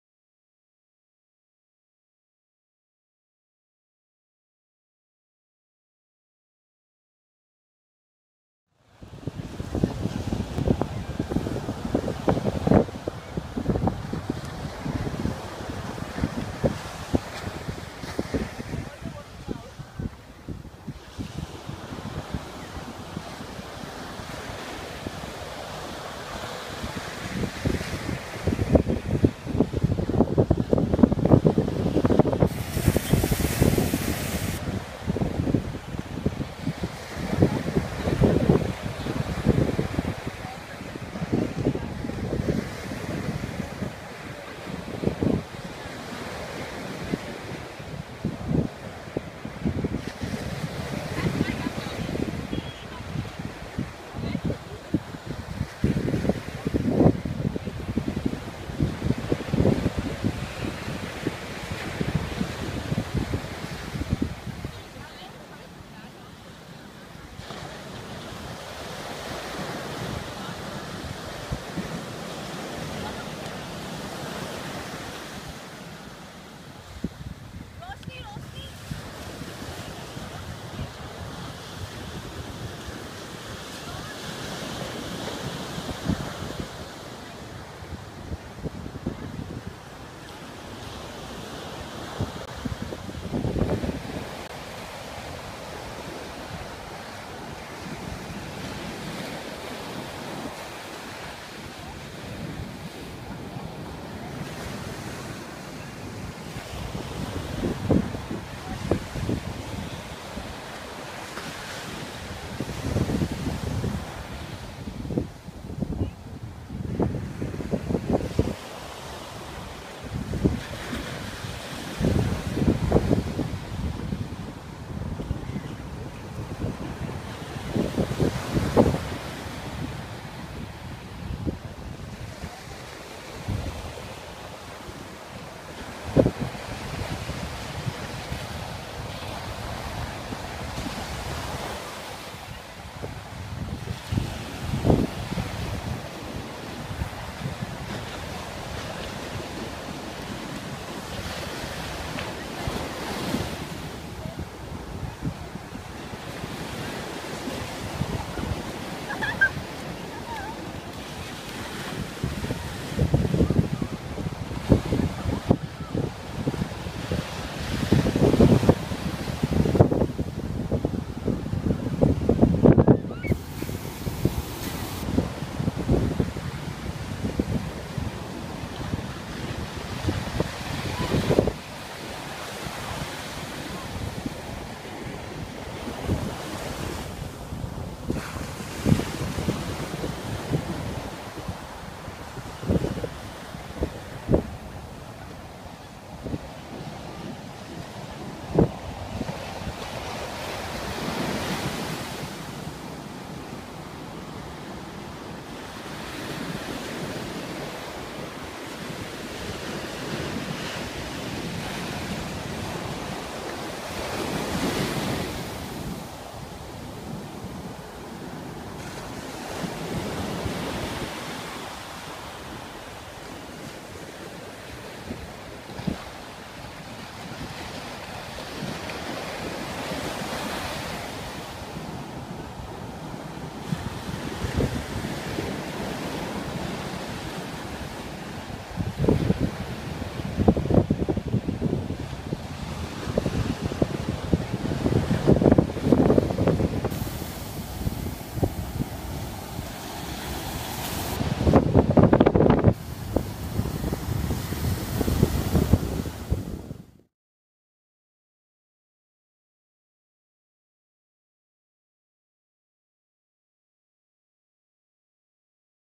Edward Elliots Beach After Sunrise,Chennai, India
Besant Nagar is one of Chennai's well known neighbourhoods, named after the famous lady theosophist Annie Besant, and adjoins the Theosophical Society. The main attraction here is Edward Elliot's beach, named after Edward Elliot, Governor of Madras.  It is a part of Chennai which is inhabited by many famous personalities and affluent citizens. Alongside are many restaurants, cafes, shops and boutiques.